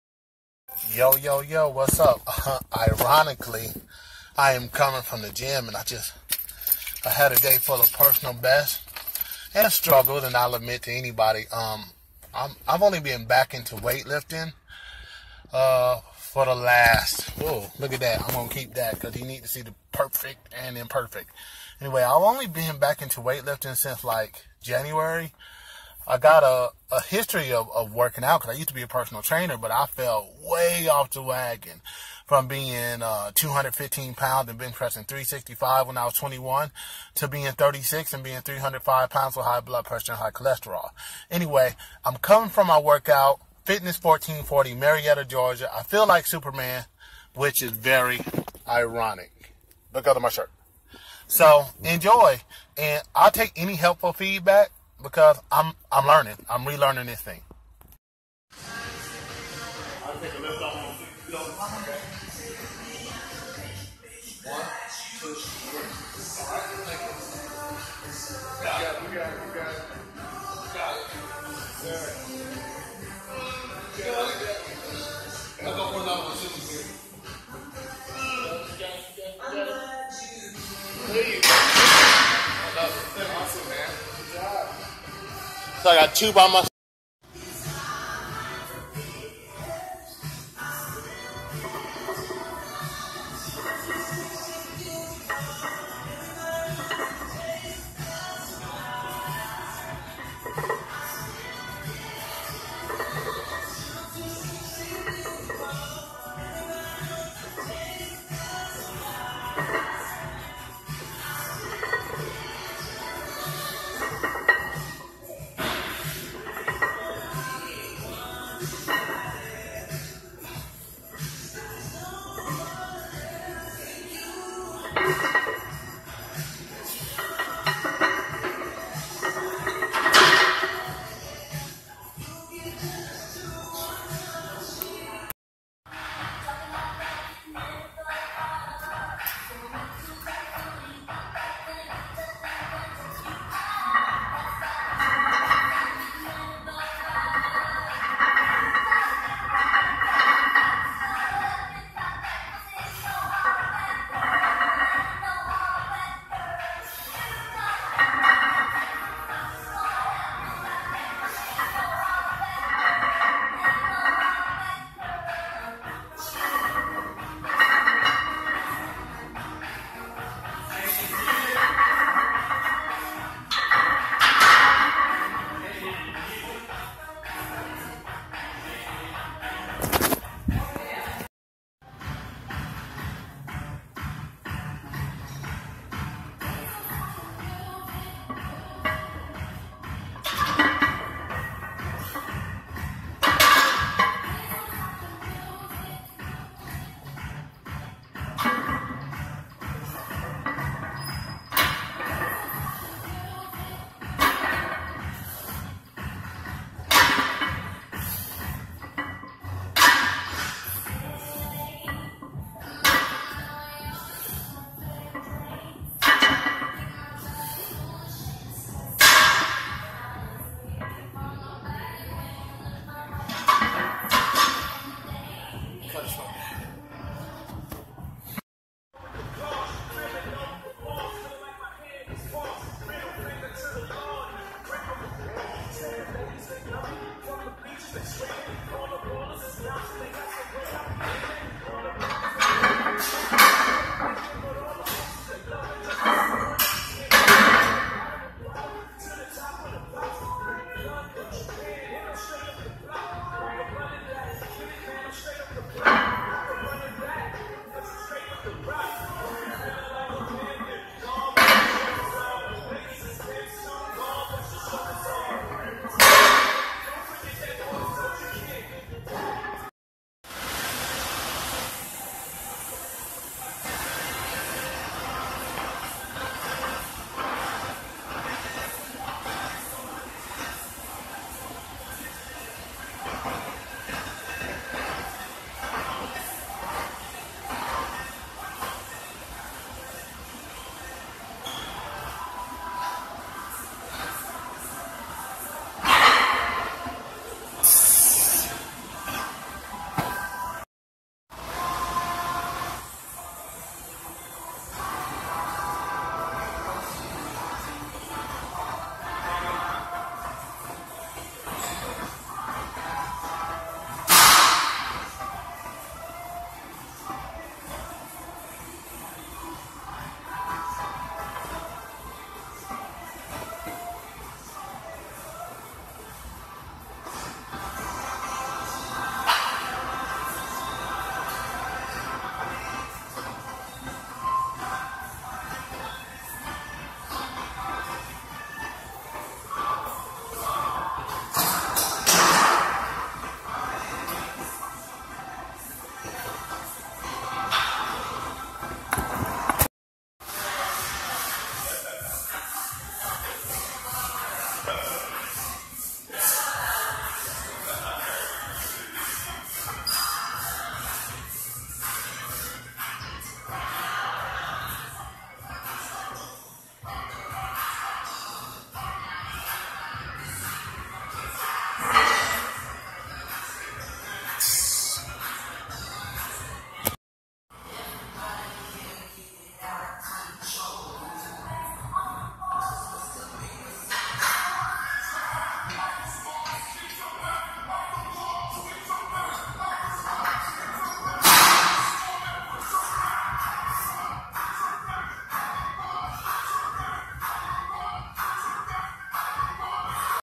Doing it big
I was at Fitness 1440 doing it again. I am still new to getting back to this... so the form needs a lot of work... but I'm giving it my all. Straight waltzilla style.